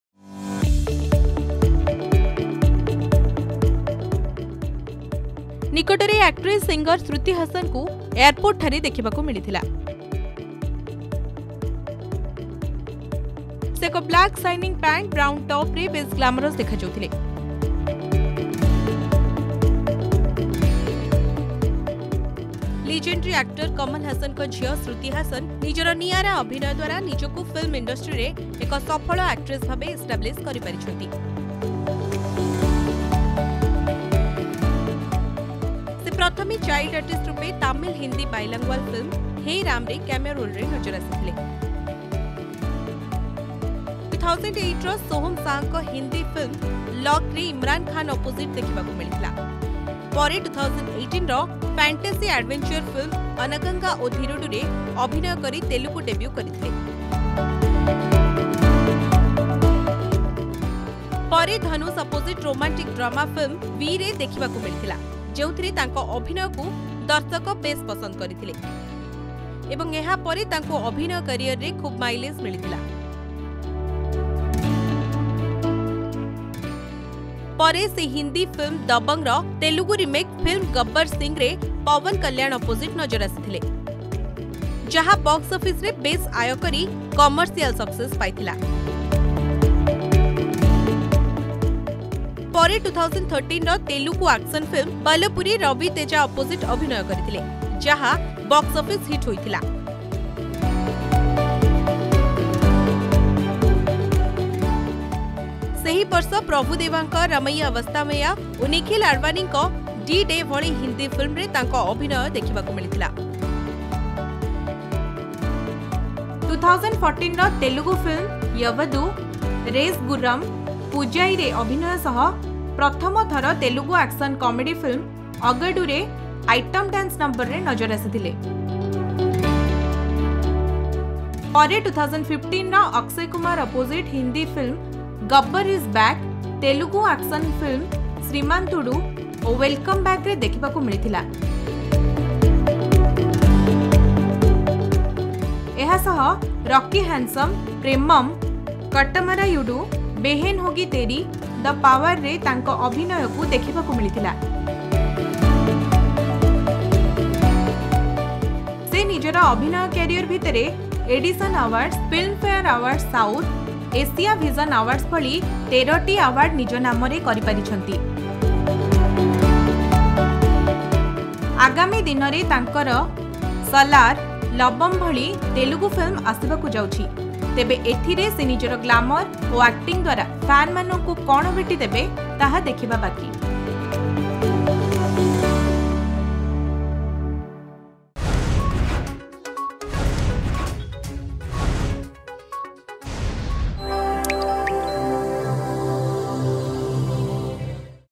Actress Shruti Haasan Spotted at the Mumbai Airport
Recently, Actress and Singer hruti Haasan was spotted at the Mumbai Airport.